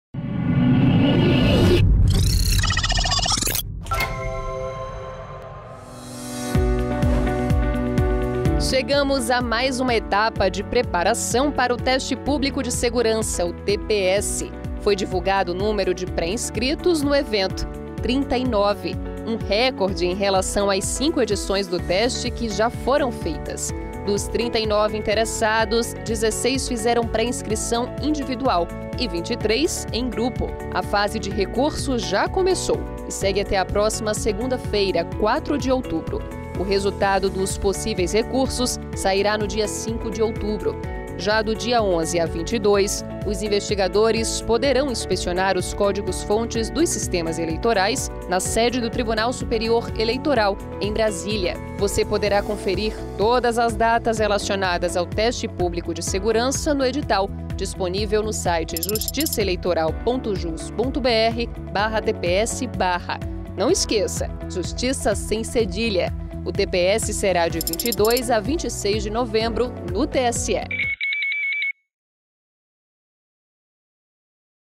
TPS tem recorde de pré-inscritos para testar a urna
Descrição – A Comissão Reguladora do Teste Público de Segurança do Sistema Eletrônico de Votação (TPS) divulgou, nesta quinta-feira (30), a lista com 39 investigadores pré-inscritos para participar da edição de 2021, que será realizada no período de 22 a 26 de novembro. O número de pré-inscrições aprovadas é recorde entre todas as edições do teste já realizadas.

Ficha técnica
Produção: Taíssa Costa e Rogério Brandão
Reportagem: Renatta Gorga
Edição: Marnie Ruas e Alexandre Fernandes
Videografismo: Weslan Rocha